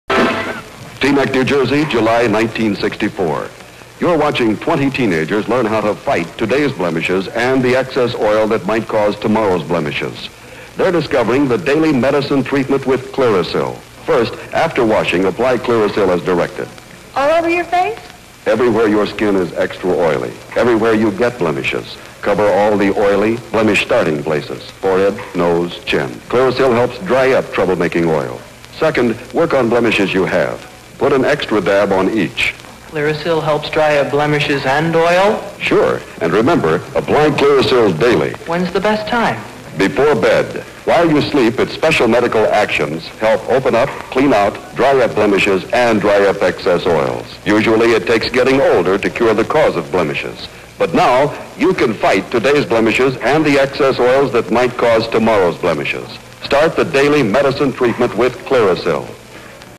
The Fight Against Blemishes
The 1964 Clearasil commercial that put Teaneck, New Jersey, on the map! Watch as an announcer shows a room full of teenagers how to prevent pimples.  To license this footage contact Global ImageWorks.

GIW 1716